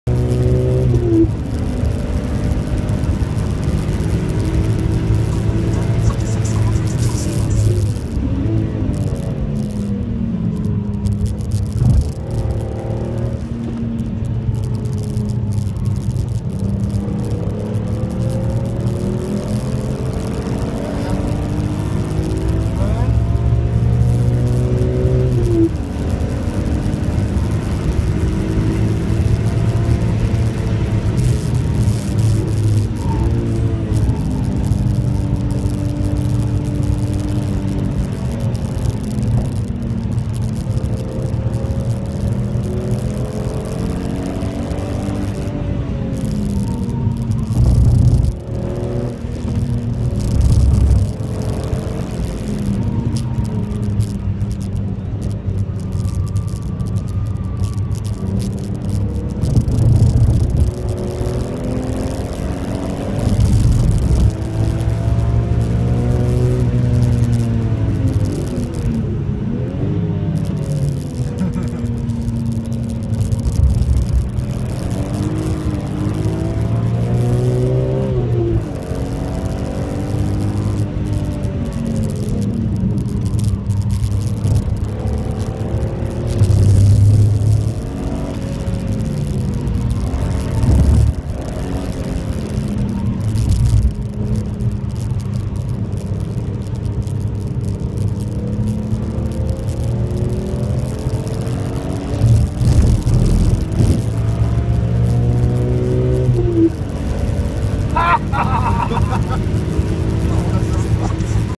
Rudskogen En runde
One round on Rudskogen in Norway.
Nice conditions. Stock car.
Testing a new suspension kit from Ôhlins. "Road and Track" for the BMW M3 e46. Great improvement from stock dampers and springs.